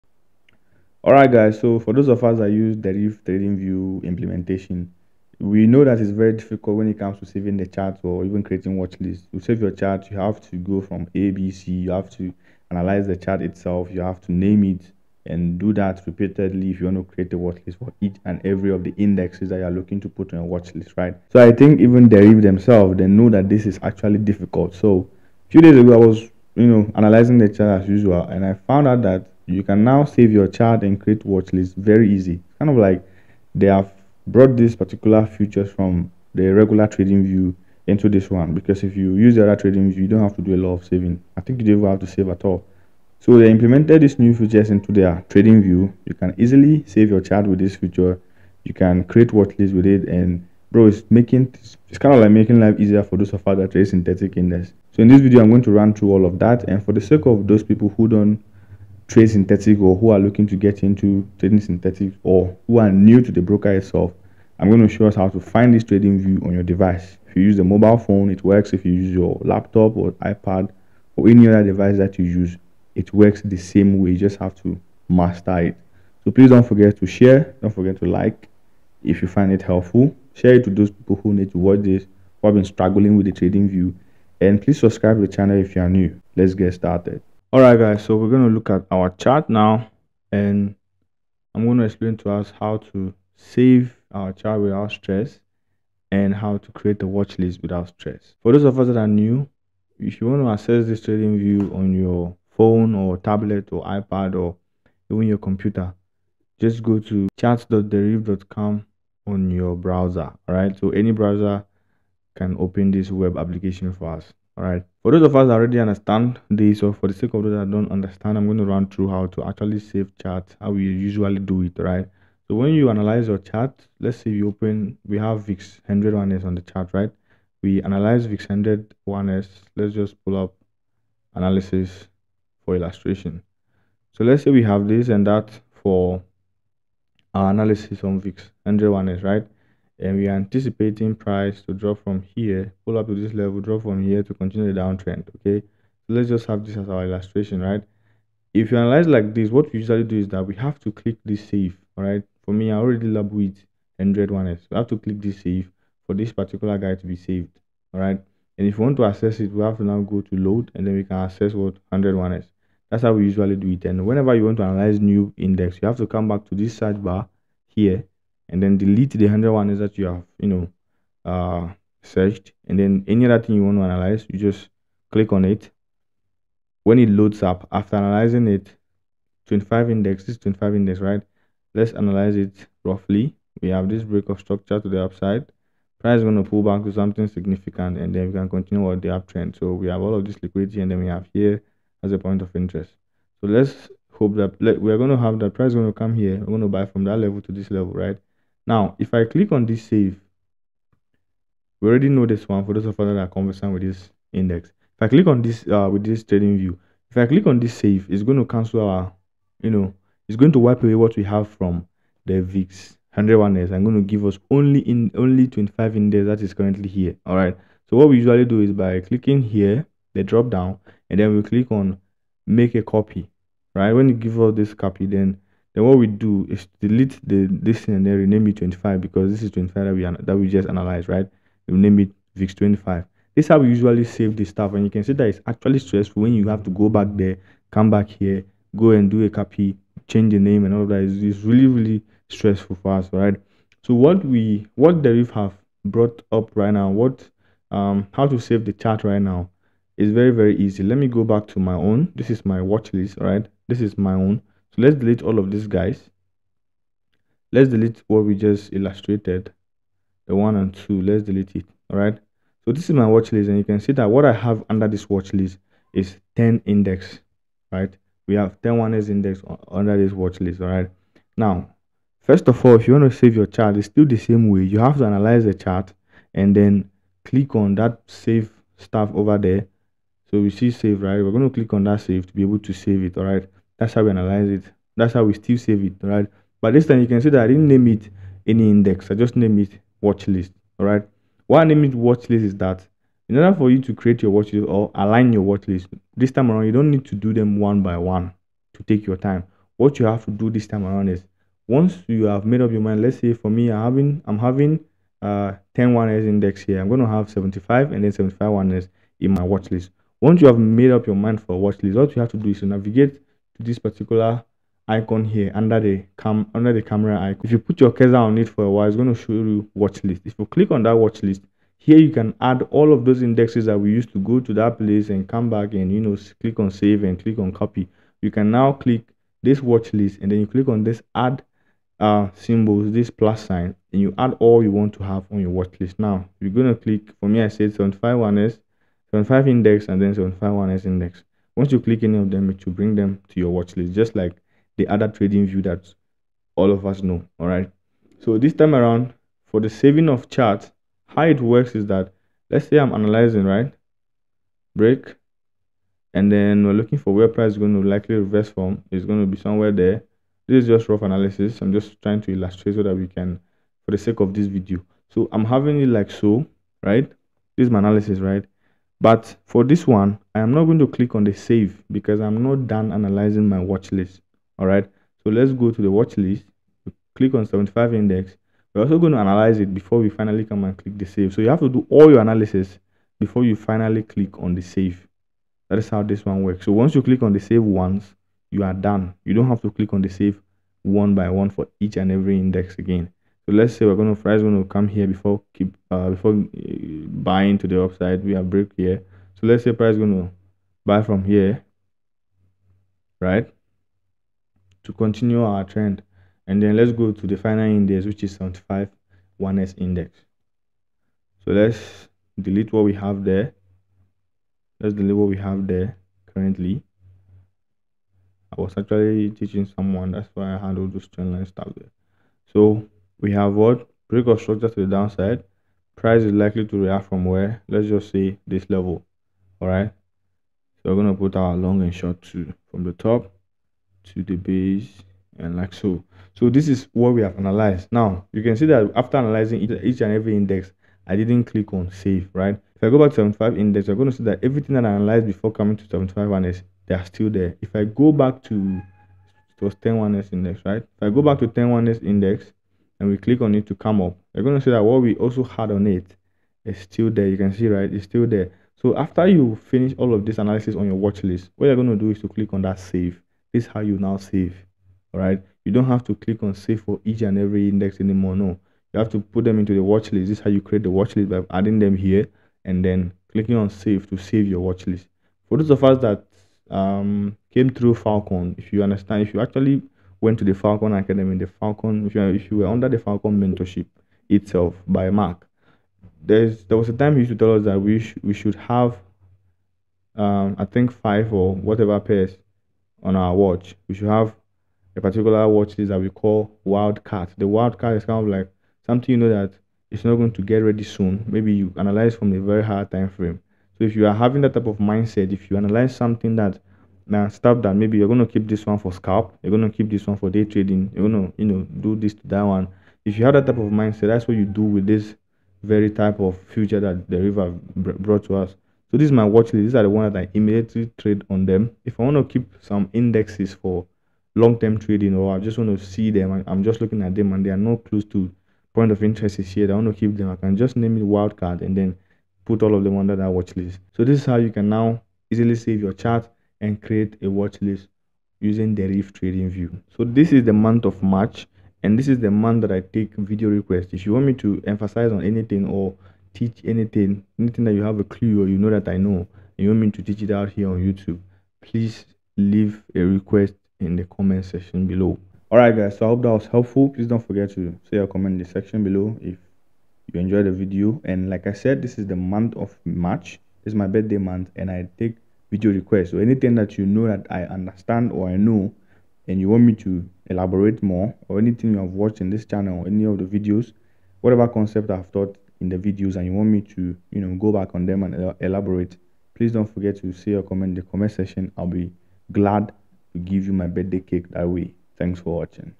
Easy Steps To Setup Deriv TradingView | Save Chart | Create Watchlist
If you trade on Deriv, watch this!!!